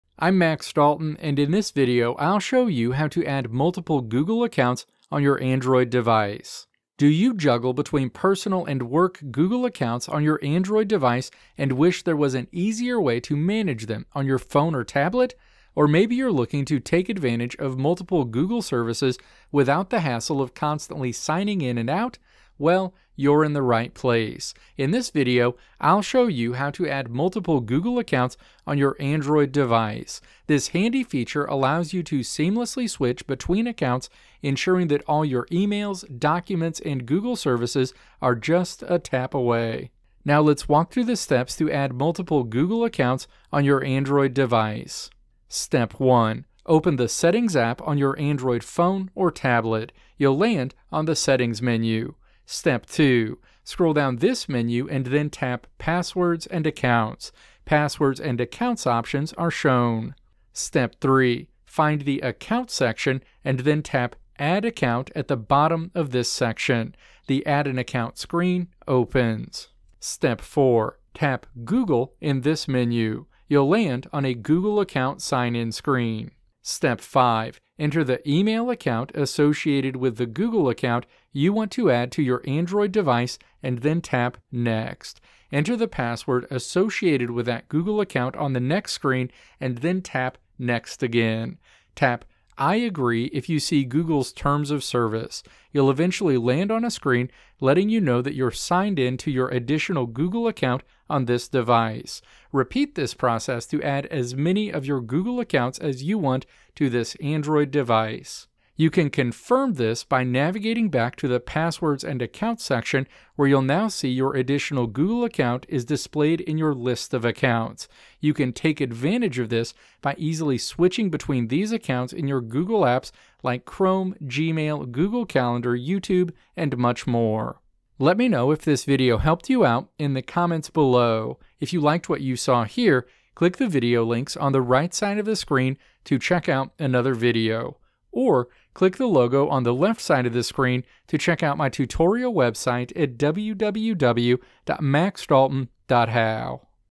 How to Add Multiple Google Accounts on Android Device
In this video I'll show you how to add multiple Google accounts on your Android device.

Timestamps:
Introduction: 0:00
Steps to Add Multiple Google Accounts on Android Device: 0:40
Conclusion: 2:07

Video Transcript:
Do you juggle between personal and work Google accounts on your Android device and wish there was an easier way to manage them on your phone or tablet? Or maybe you're looking to take advantage of multiple Google services without the hassle of constantly signing in and out? Well, you're in the right place! In this video I'll show you how to add multiple Google accounts on your Android device. This handy feature allows you to seamlessly switch between accounts, ensuring that all your emails, documents, and Google services are just a tap away.

Now let's walk through the steps to add multiple Google accounts on your Android device.

Step 1. Open the Settings app on your Android phone or tablet. You'll land on the settings menu.

Step 2. Scroll down this menu and then tap "Passwords and Accounts." Passwords and accounts options are shown.

Step 3. Find the Account section, and then tap "Add Account" at the bottom of this section. The Add an Account screen opens.

Step 4. Tap "Google" in this menu. You'll land on a Google account sign in screen.

Step 5. Enter the email account associated with the Google account you want to add to your Android device, and then tap "Next." Enter the password associated with that Google account on the next screen, and then tap "Next" again. You'll eventually land on a screen letting you know that you're signed in to your additional Google account on this device. Repeat this process to add as many of your Google accounts as you want to this Android device.

You can confirm this by navigating back to the Passwords and Accounts section, where you'll now see your additional Google account is displayed in your list of accounts. You can take advantage of this by easily switching between these accounts in your Google apps, like Chrome, Gmail, Google Calendar, YouTube and much more.